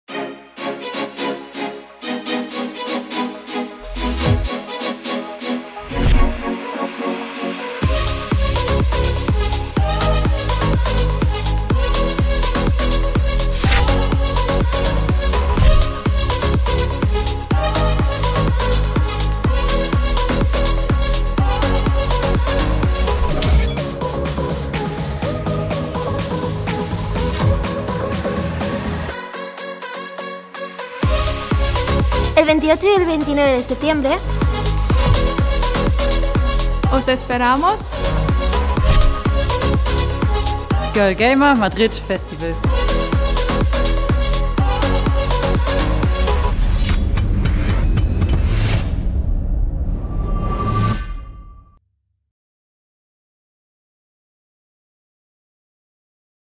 Llega el GIRL GAMER FESTIVAL al Movistar Esports Center
El 28 y 29 de septiembre, no te pierdas la parada Europea de GIRLGAMER que Movistar Riders organiza por primera vez en Madrid. ¡Habrá League of Legends, CS:GO y muchas sorpresas!

¿Quieres estar al tanto de todas las noticias de nuestro club?